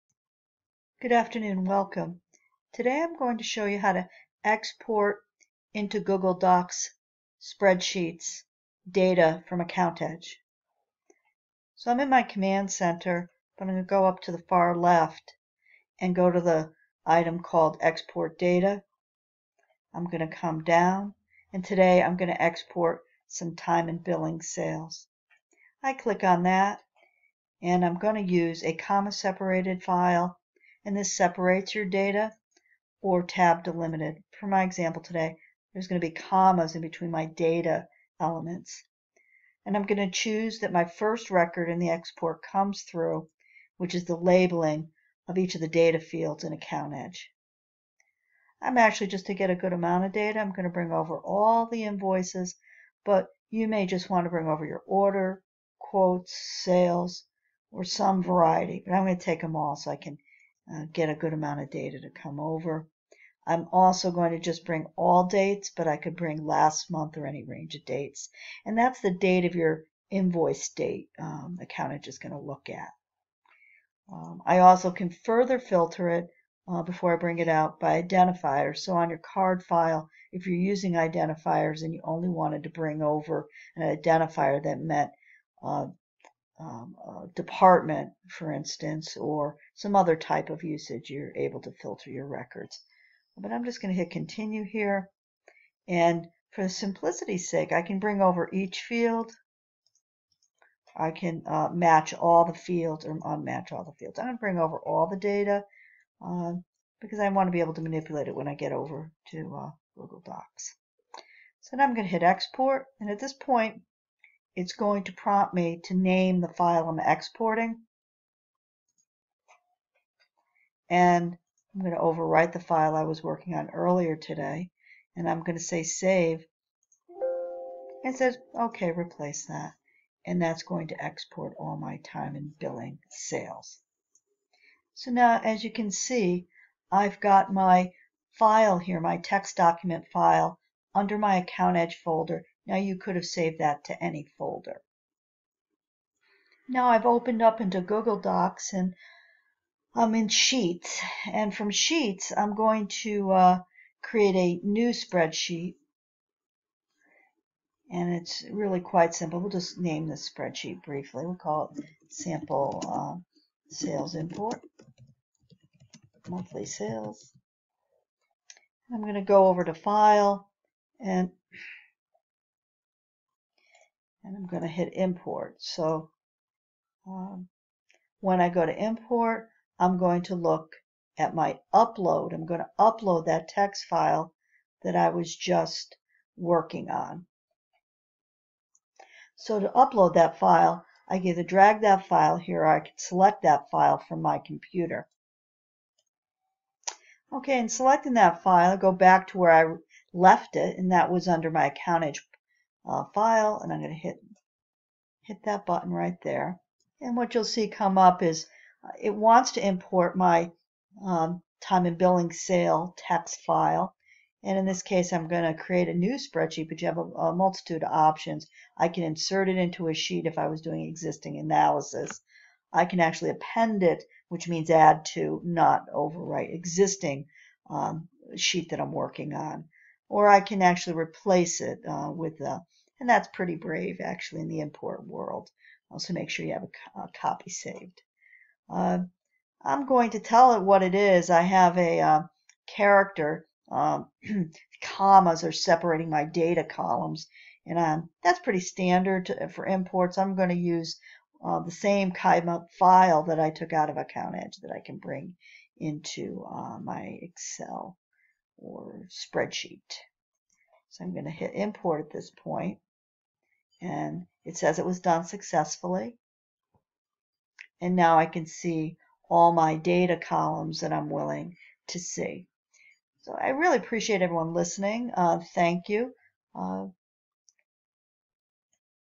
Part 10 Export Sales to Google Sheet
Instructional video to export then import into google Docs AccountEdge